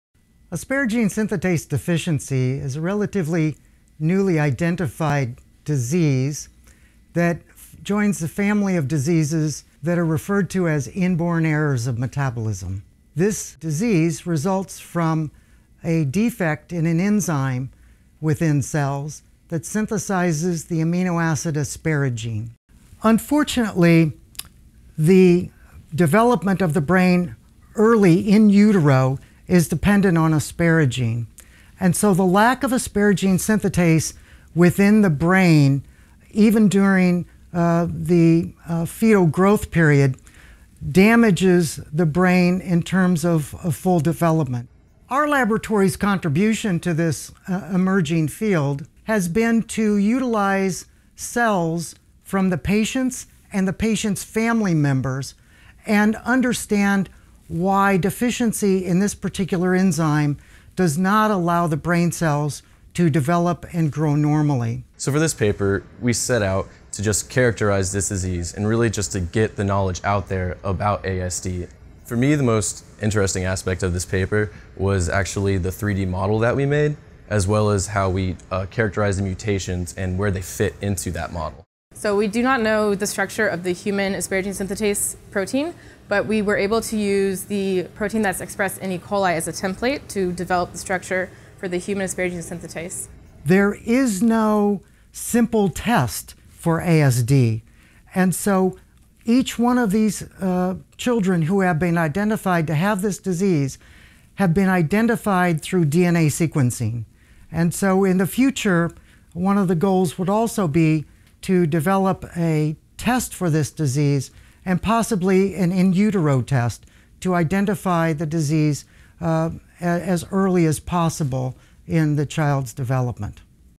UF Health researchers target brain cell disorder
University of Florida Health researchers are using cells from patients and patients’ family members to understand why an enzyme deficiency does not allow brain cells to develop and grow normally. The disorder, known as asparagine synthetase deficiency (ASD) , affects children and was only identified several years ago.  Biochemistry professors Michael S. Kilberg, Ph.D., and Robert McKenna, Ph.D., and two doctoral students recently published a paper about ASD’s function, structure and role in disease.